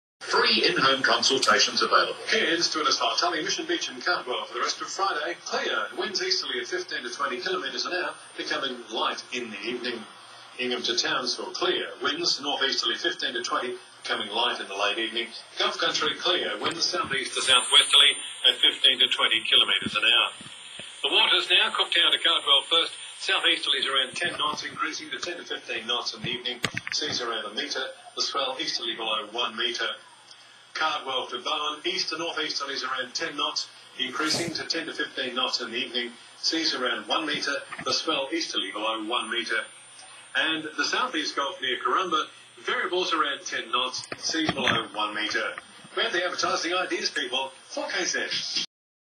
4KZ Radio Weather Forecast #51
12th September 2014
9pm Weather Forecast

Here is more of these NQ Radio Weather Forecasts. These next few or so videos have another radio station playing the weather forecasts and best of all, one has the 4KZ Radio Channel and the Pocket Radio has the mystery channel.